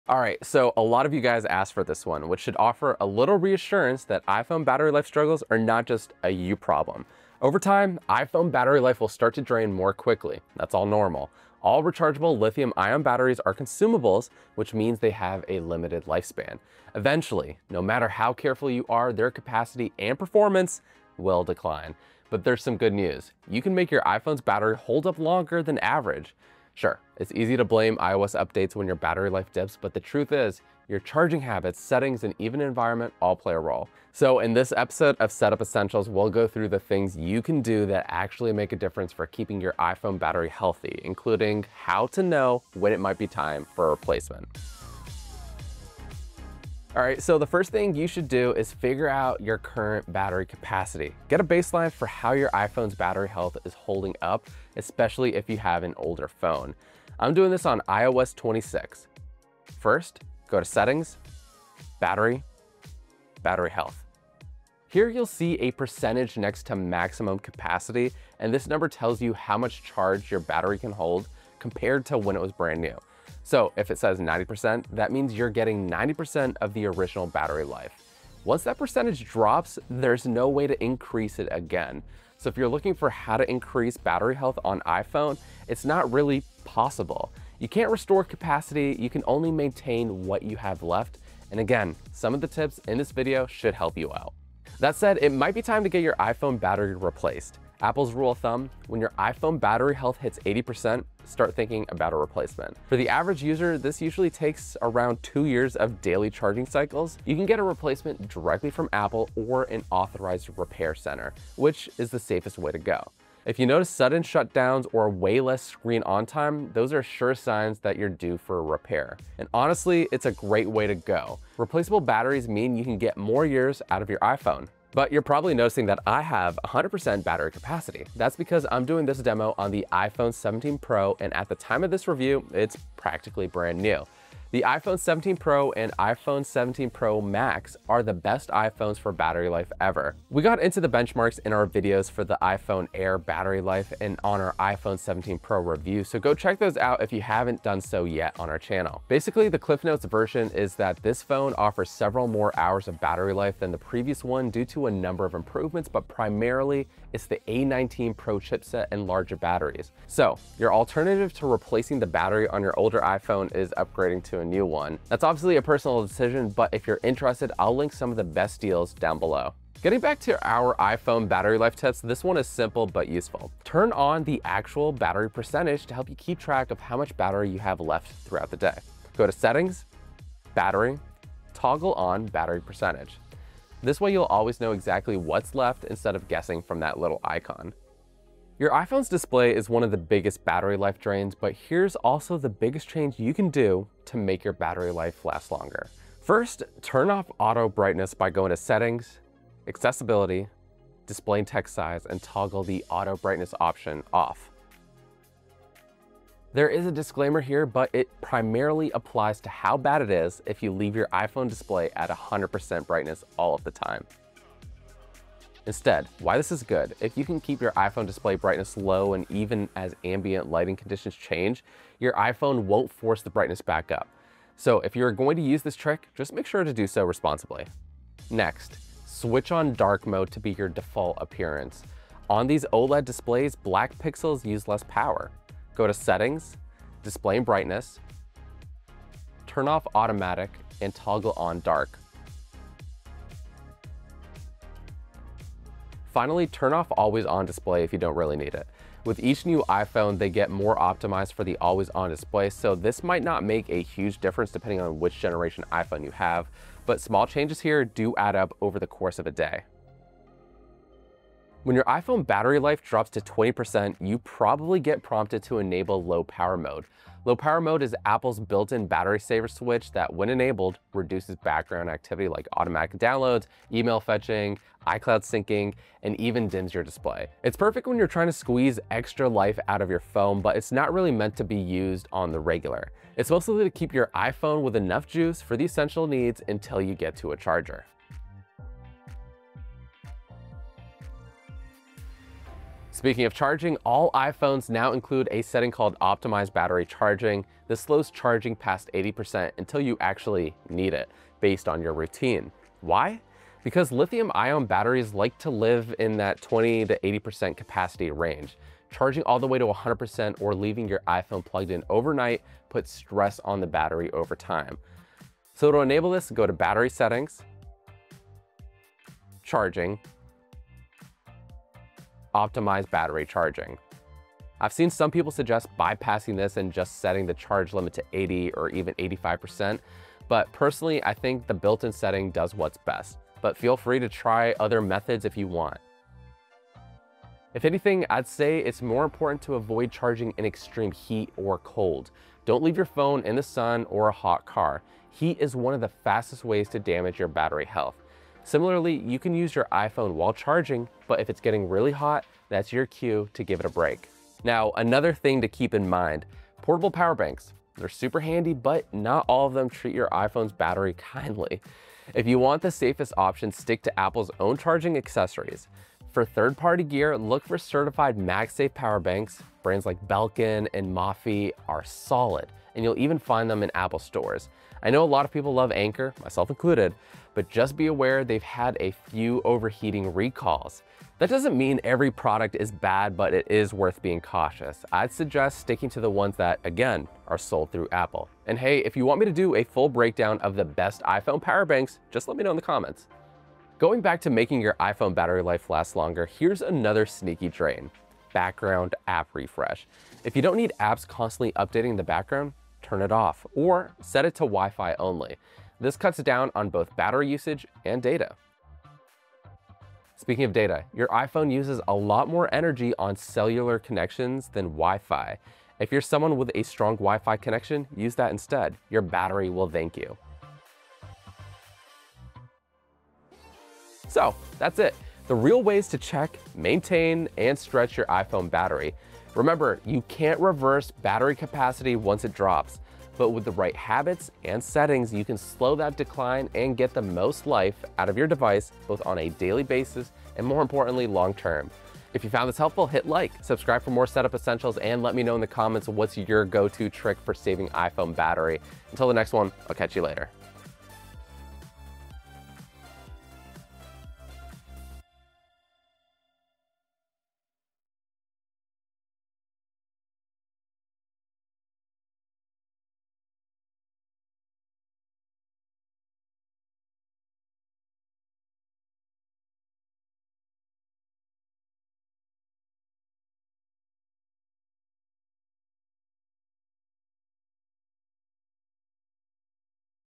How to Make Your iPhone Battery Last Longer (Settings That Matter) | Setup Essentials
Struggling with iPhone battery life? In this episode of Setup Essentials, Paul shows you how to check the battery on your iPhone, what iPhone battery capacity really means and the iPhone settings for battery life that actually make a difference. He covers how to check battery life on an iPhone, tips on how to maintain iPhone battery health long-term, and how to know when it’s time for an iPhone battery replacement since you can’t truly increase battery health on an iPhone once it declines.
But what can you do to make it last longer? By the end, you’ll know exactly how to adjust your habits and settings to squeeze the most out of your iPhone’s battery.
💬 Got a favorite tip for saving battery? Drop it in the comments!

0:00 - How to save battery on iPhone
0:44 - How to check battery health on iPhone
1:04 - What is Battery Capacity in iPhone?
1:34 - Time for iPhone battery replacement?
2:04 - Best iPhone for battery life
2:49 - How to see your battery percentage
3:08 - iPhone display settings to save battery
4:21 - What is iPhone Low Power Mode?
4:52 - Optimize Battery Charging
5:54 - iPhone Battery Packs
6:33 - Background app refresh
6:49 - Use Wi-Fi instead of mobile data
7:02 - iPhone settings for battery life